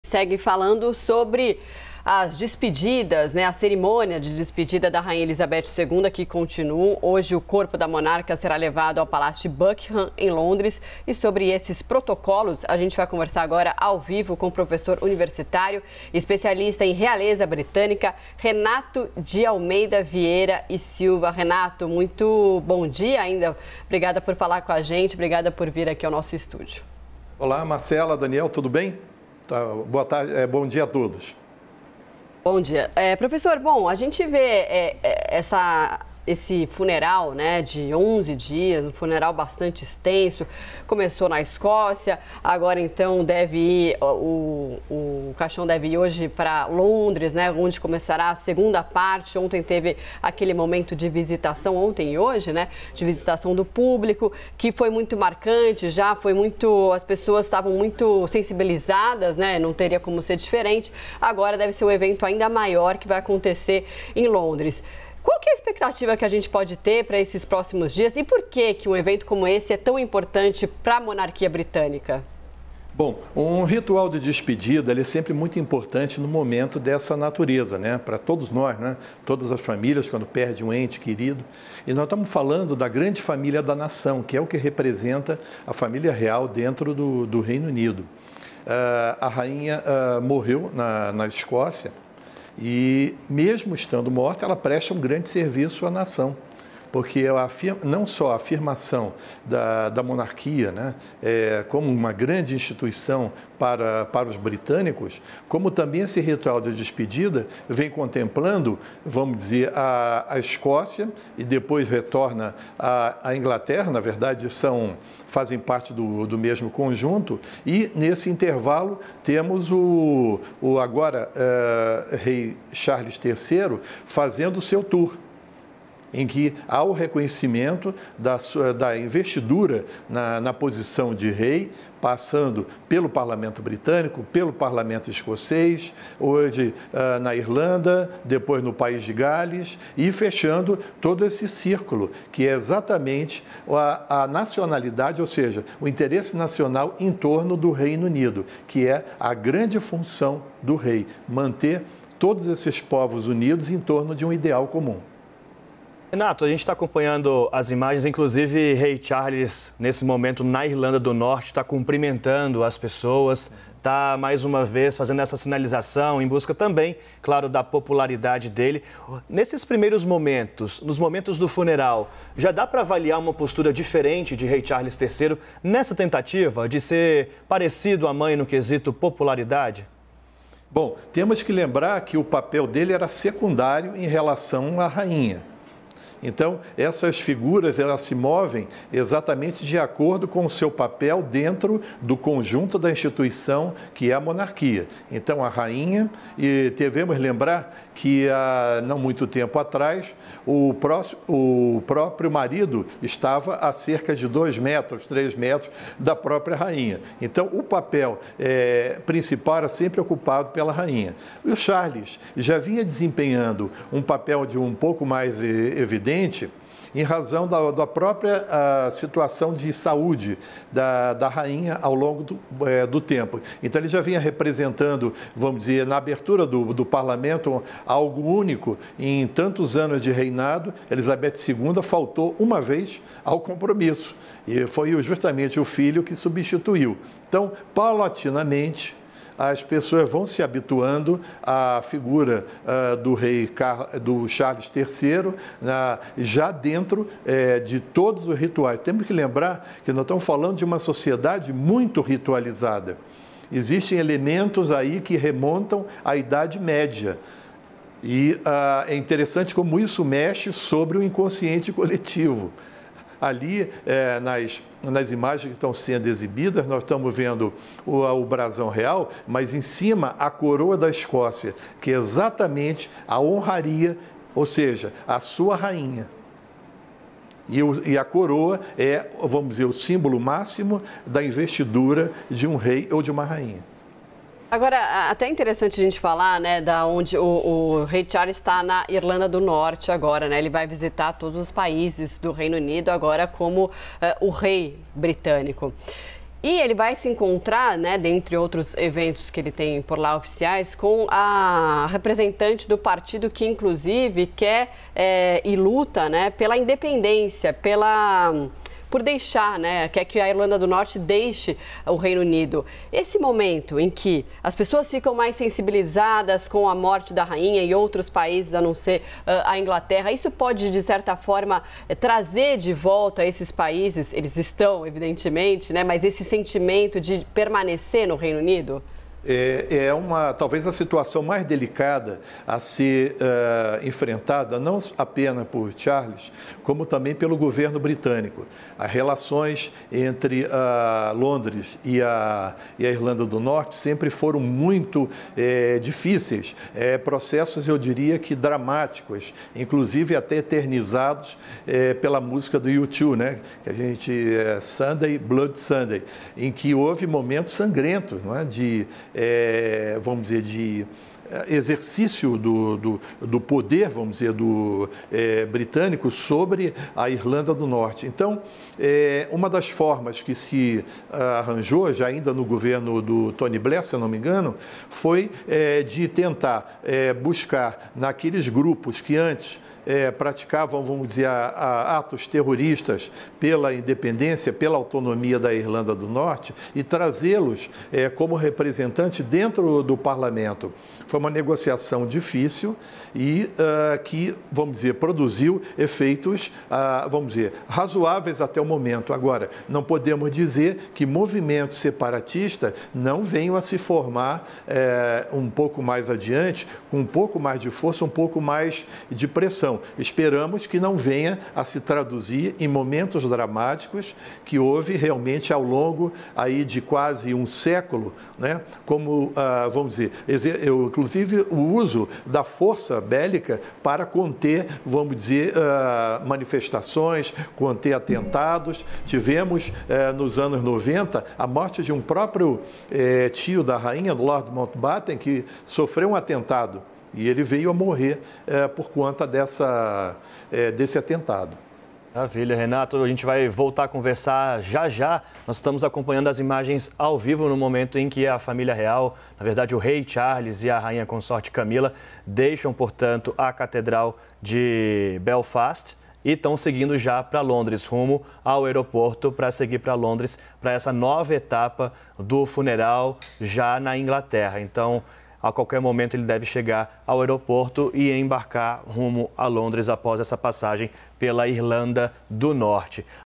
Grupos separatistas na Irlanda do Norte podem ganhar força, diz professor | LIVE CNN
Em entrevista à CNN nesta terça-feira (13), Renato de Almeida Vieira e Silva, professor universitário e especialista em realeza, disse que os movimentos separatistas na Irlanda do Norte podem ganhar força após morte da rainha Elizabeth II.